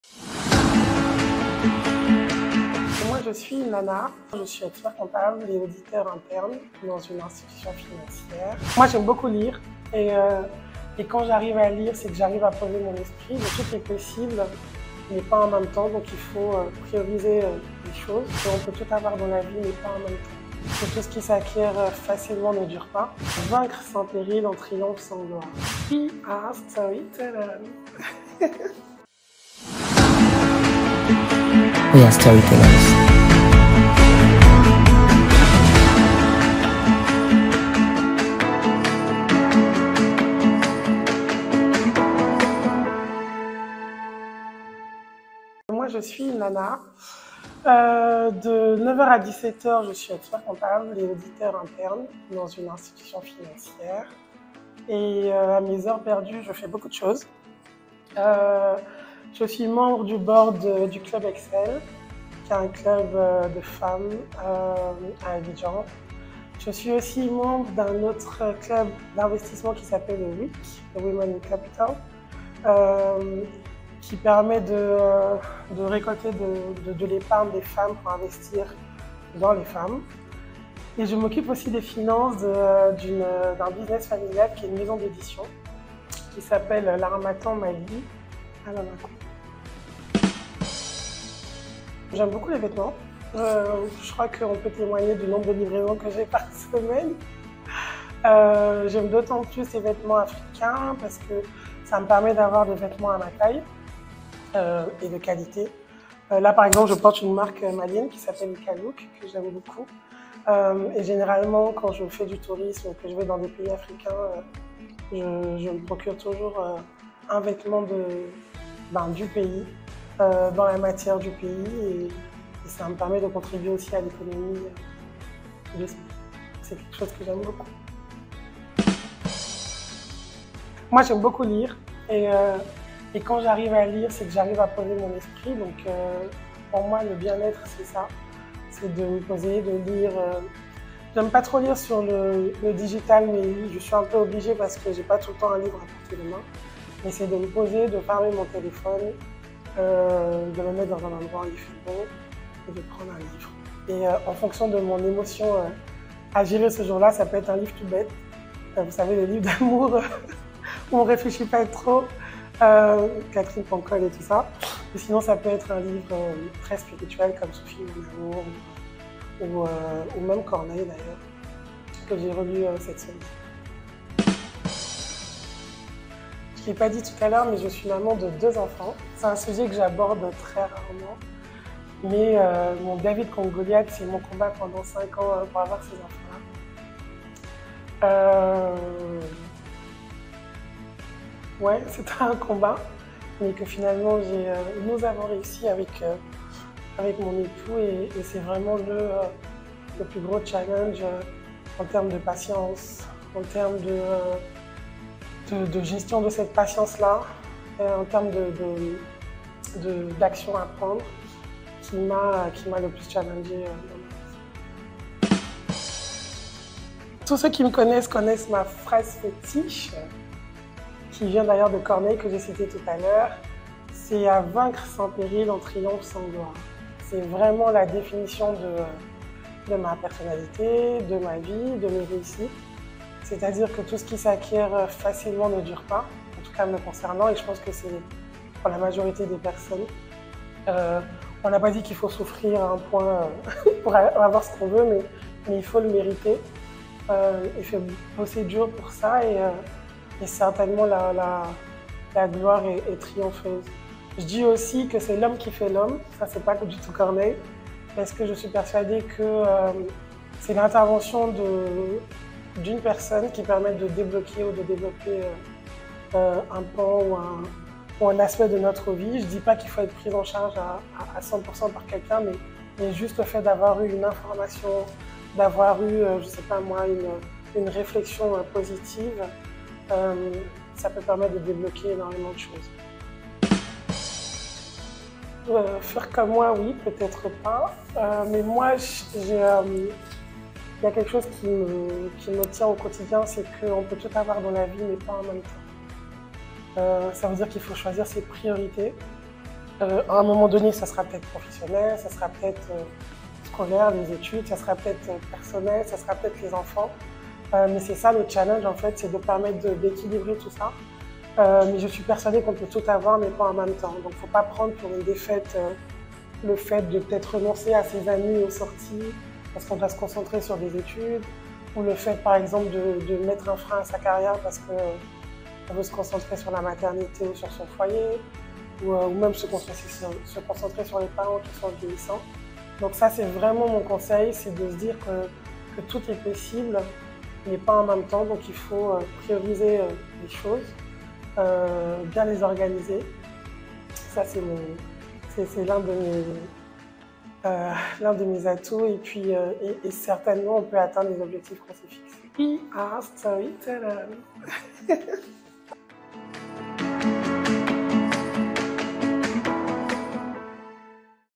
Tout est possible mais il faut apprendre à prioriser les choses - Nanna SYLLA COULIBALY
« Vaincre sans péril, on triomphe sans gloire. Tout est possible mais il faut apprendre à prioriser les choses » - Nanna SYLLA COULIBALY
Expert-comptable diplômée

WE ARE STORYTELLERS
Projet de capsules engagées par INNSAEI et le Think Tank Club Nanan